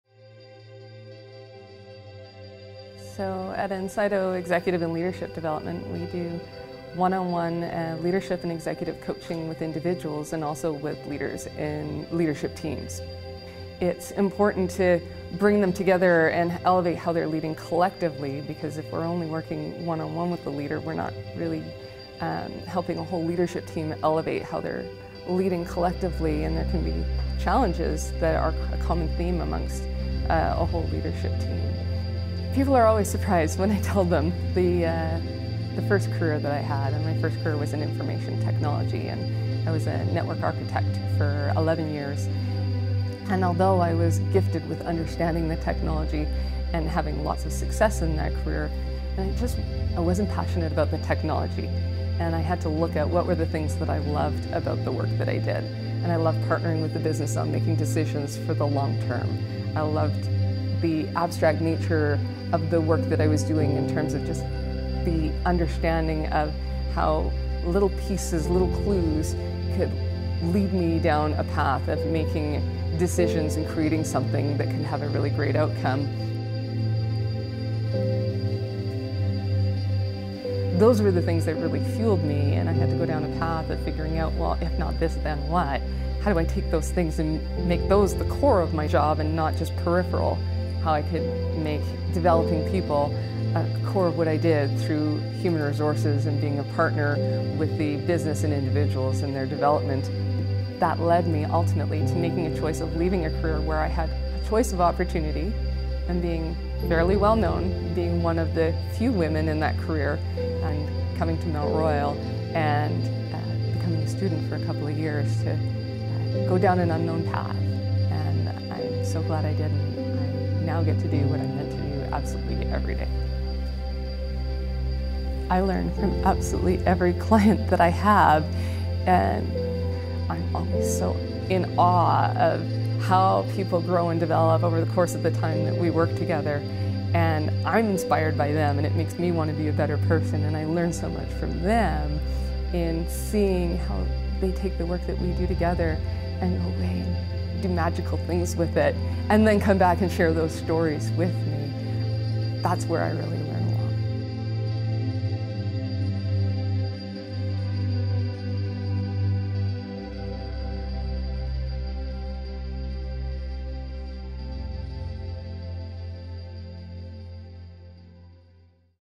Jenn Lofgren, Outstanding Alumni Award Recipient
Business Administration -- Human Resources Diploma, 2006

Jenn Lofgren is the driving force behind Incito Leadership and Executive Coaching, a company that inspires smart, creative and bold leaders.